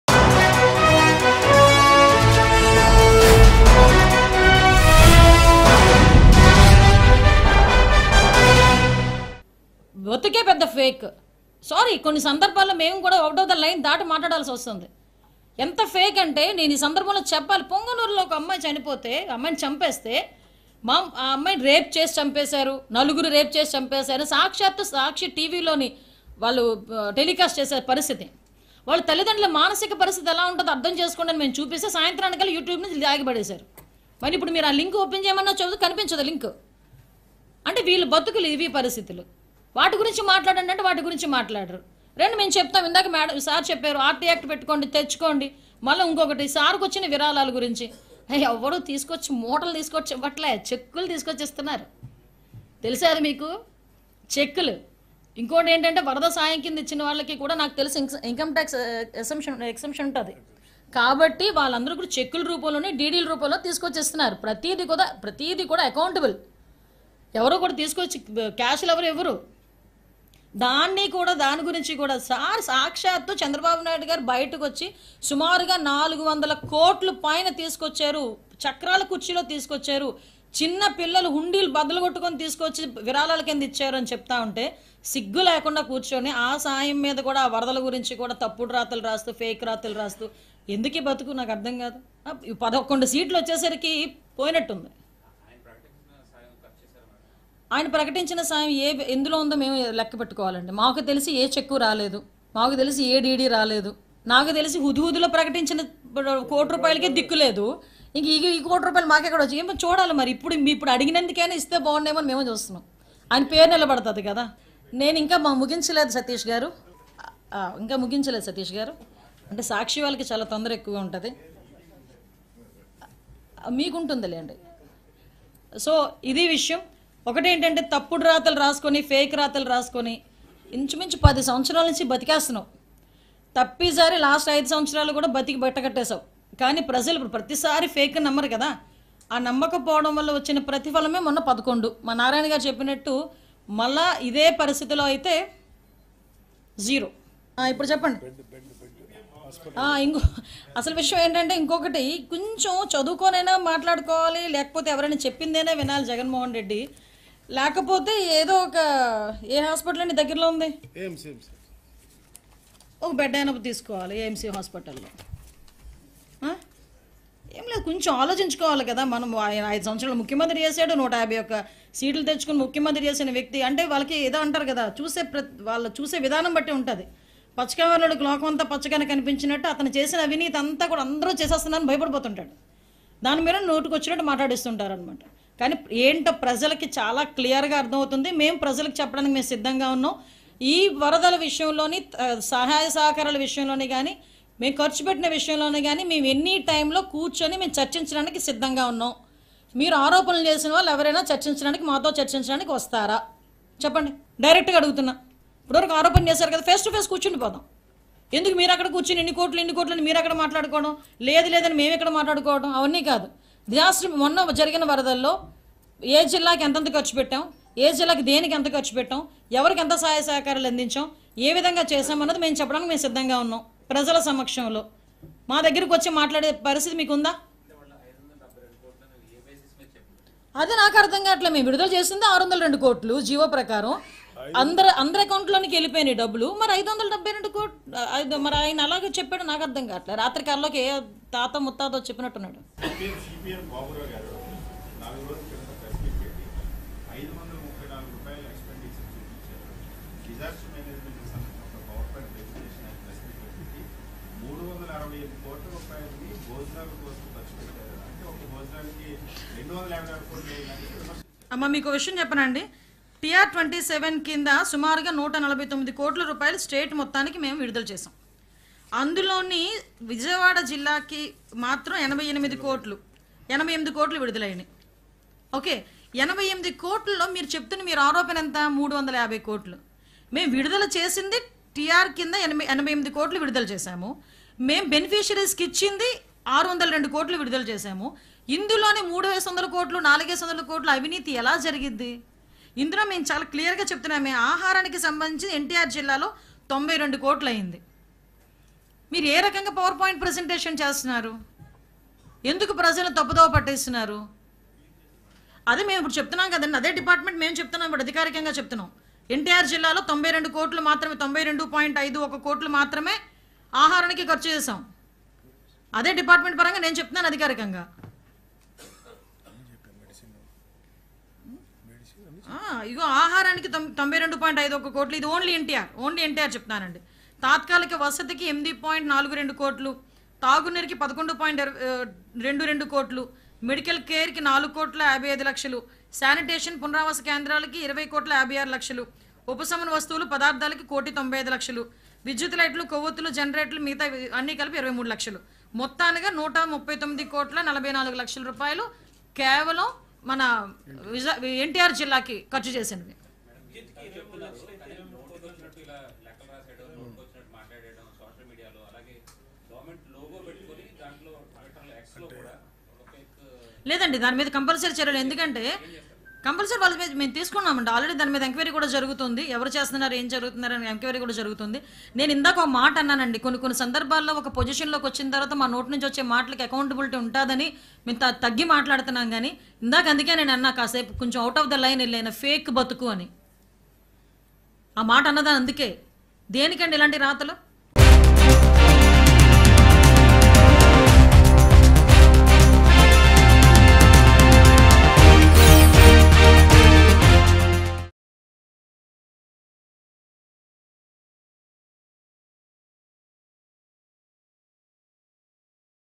Home Minister Vangalapudi Anitha Open Challenge To YCP | TDP Vs YCP | AP Politics | Mango News
Home Minister Vangalapudi Anitha Open Challenge To YCP. She Alleges YS Jagan-Related Media is Publishing Fake News About AP Government.